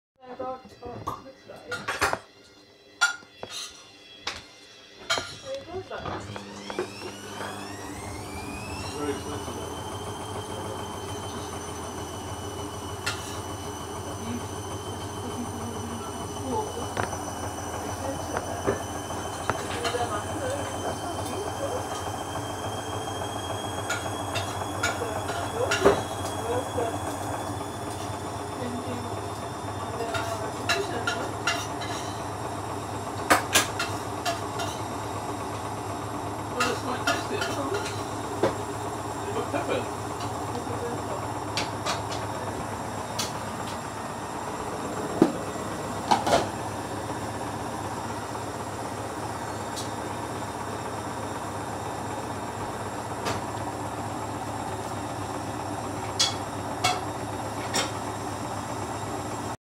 Hoover dynamic coloureds 40 c sensitive care part 12
We are on  Fifth spin  and we got a sudslock spin. No final rinse , final spin  because I could not film it.